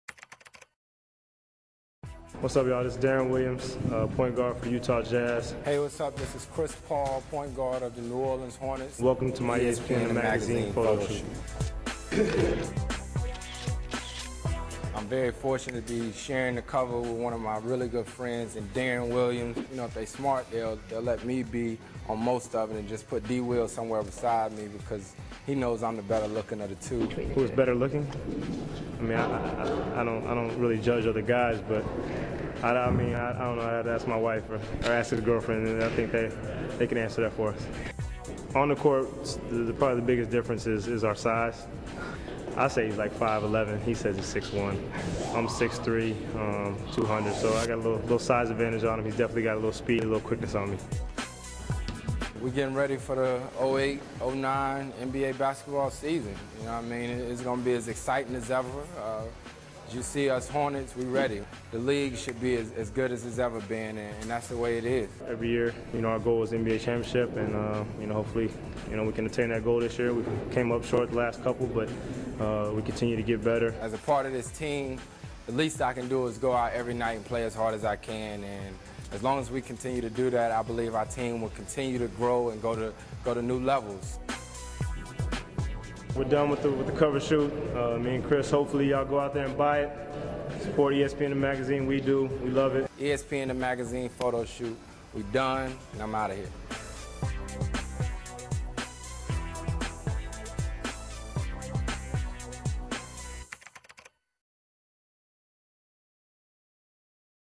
Deron Williams and Chris Paul photo shoot with ESPN the Mag
Deron Williams and Chris Paul photo shoot with ESPN the Magazine.